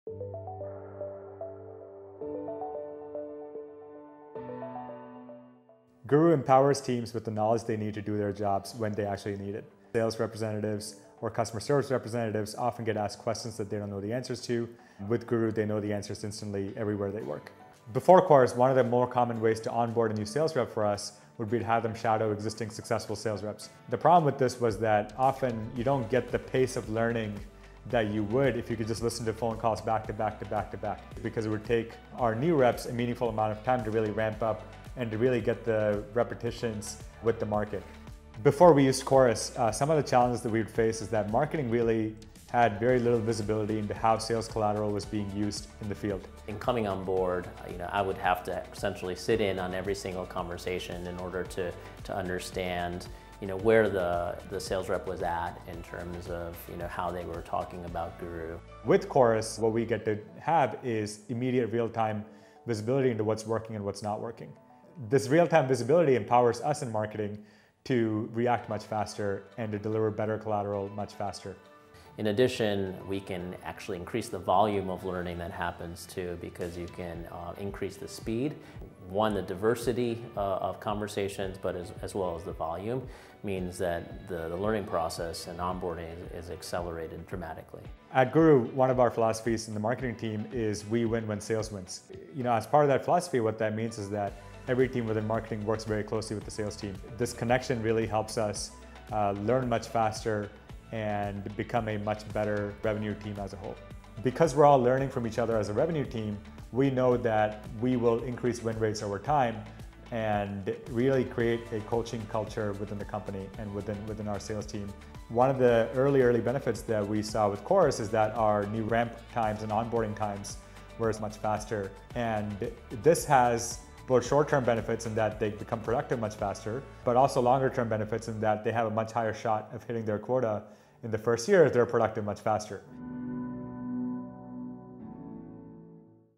Chorus.ai + Guru
Learn how Chorus.ai helps Guru dramatically accelerate ramp times, create a coaching culture, and speed up learning across sales and marketing for an aligned revenue team. Guru also gained real-time visibility into how marketing collateral is used by their sales team to learn what's working in the field.